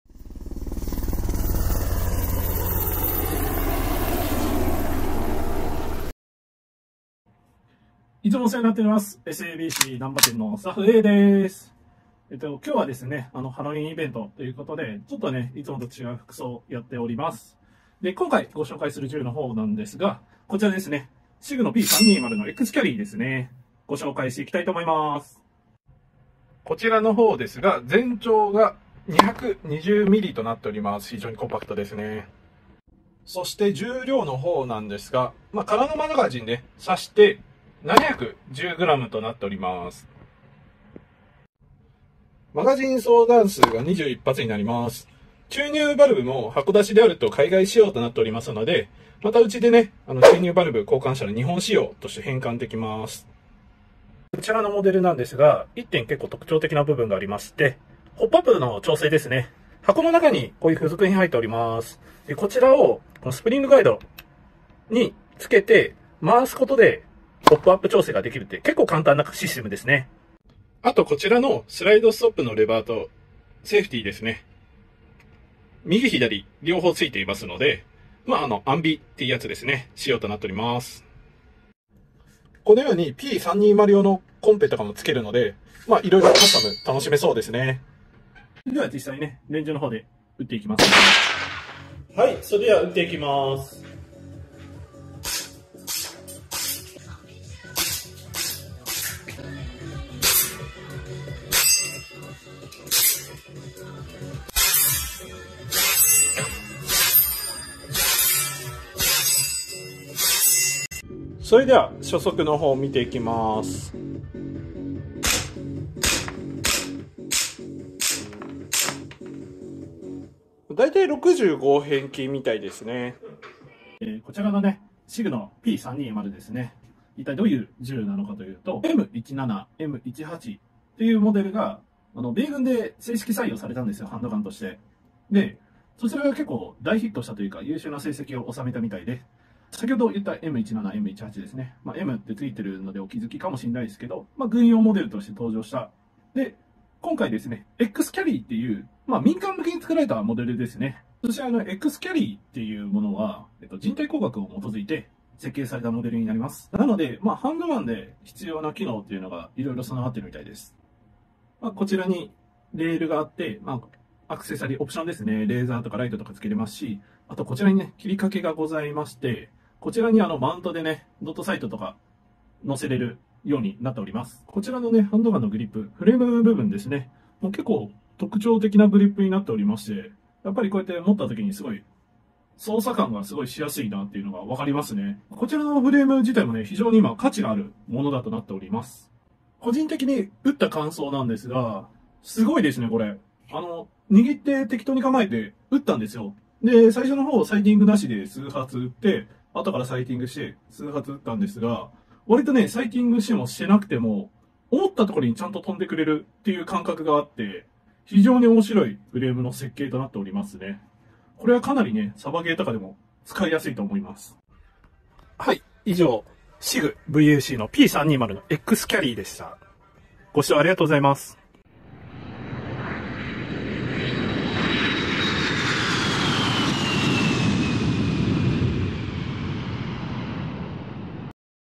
【もはやサイティング不要!】SIG/VFC P320 X CRRY GBBハンドガン
いつもお世話になっております
ＳＦＢＣ難波店スタッフＡです！
SIG/VFC P320 X CRRYについてのご紹介なります
最近民間モデルどんどん出てて流行りを感じますね＾＾
フレームが大人気モデルだけあってサイティングしなくても10m
ほどの距離であれば思ったところに当たるのが非常に面白いです
正直当てやすすぎてビックリしてます！
ＳＩＧ好きな方これは買いです

【SFBCリンク集】
SFBC難波店Twitter(★本YouTubeはこちらで撮影してます)

 / sfbc35405327

SFBC日本橋店Twitter

 / sfbc0116